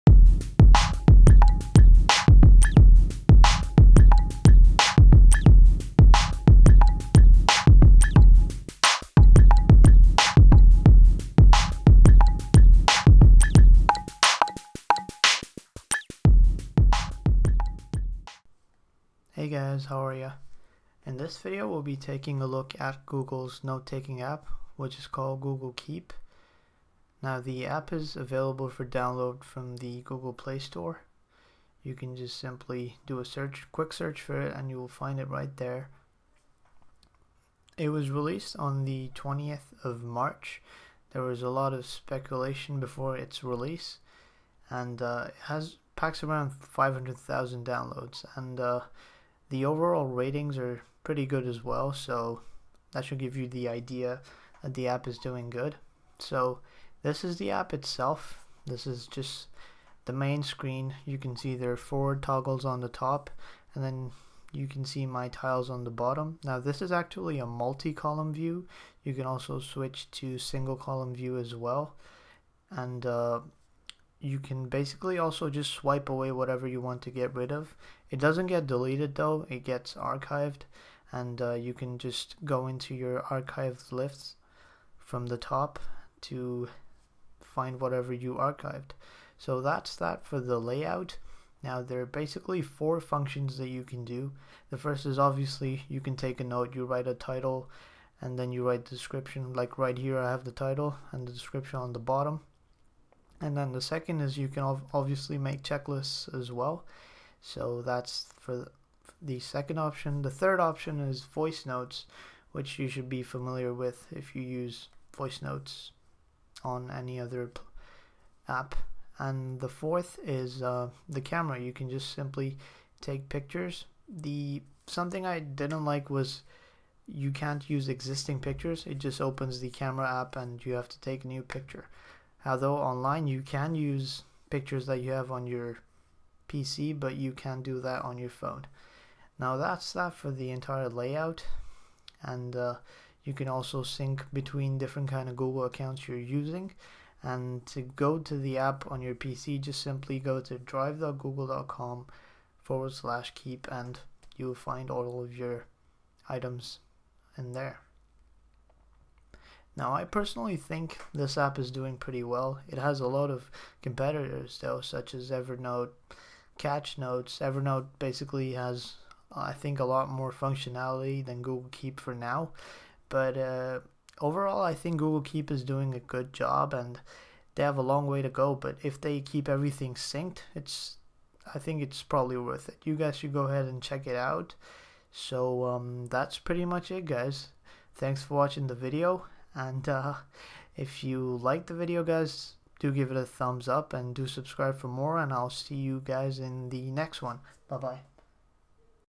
Google Keep
Follow me and ask!

In this video i take a look at Google's note taking app called Google Keep
Its available for download from the Google :-

It was released on the 20th of March and after several leaks and spottings it finally did come to be true!
If you guys love taking quick notes and need a handy note taking app, then look no further than google keep.
It definitely does have a lot of competition such as evernote, catch notes and a lot more but its ability to sync across across all devices and its also integrated into google drive makes it stand out